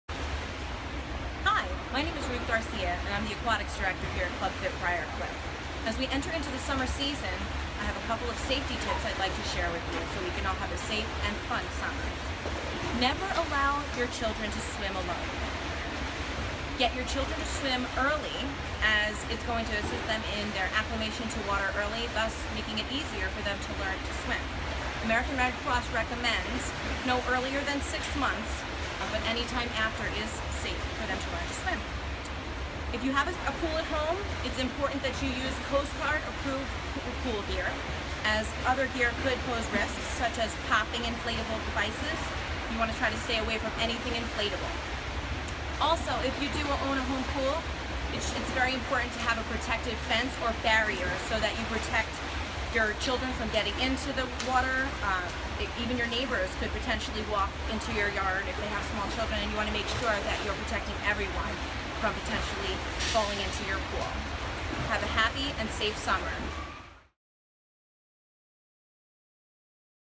Swimming and Water Safety tips- Club Fit, Briarcliff Manor, NY
We want you to have a safe and fun Memorial Day and a great Summer to follow. Here are some essential safety tips for water fun in the sun!

If you are a pool owner, make sure your pool is surrounded by an adequate fence or barrier to protect your family as well as potential wanderers.

Children should start learning to swim around 2.5 years of age. We recommend starting even sooner with infant/parent classes to get them acclimated to the water. (American Red Cross does not recommend swim lessons before 6 months of age.)

If swimming in an unsupervised area, make sure you have another person with you. Children should never be unattended.

Don’t consume alcohol while swimming. It impairs ability, judgement and coordination.

Use Coast Guard approved gear and non-inflatable pool toys and swim aids. Inflatable floatation devices pose an increased risk of drowning as they are prone to popping. We also do not recommend using mermaid-like/uni-fin fins. This new trend is cute, but not necessarily safe as it positions the body in an unnatural way and has the potential to cause a distressed swimmer.

Be sure to protect yourself from harmful UV rays. Wear an SPF or light colored clothing that covers the body and remember to take frequent breaks from the direct sun.